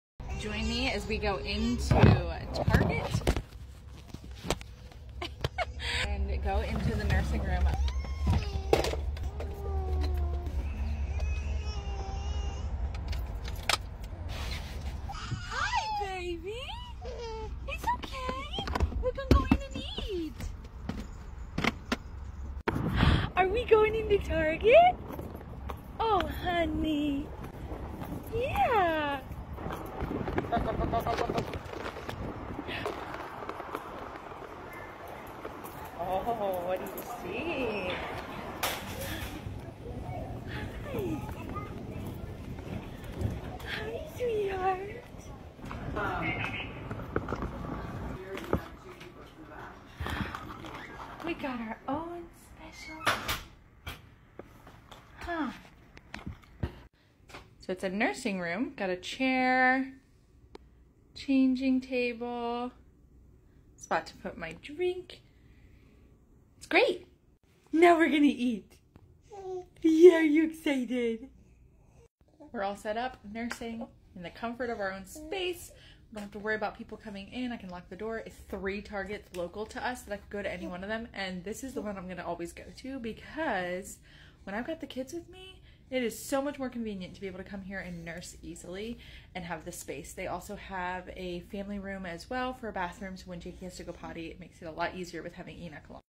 Nursing Rooms at Target
These should be in every big establishment! Makes life with kids so much easier! Thanks @target